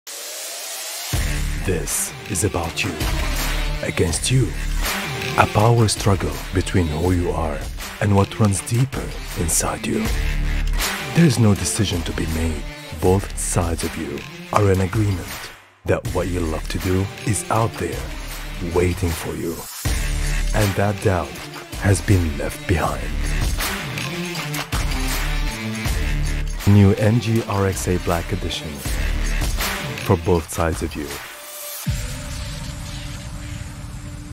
The 2022 MG RX8 Black Edition
The lineup of MG has never looked this attractive. Your favourite 7-seater, the MG RX8 is now available in an exclusive Black Edition.

Units are limited so hurry and reserve yours from the nearest MG dealer in your area.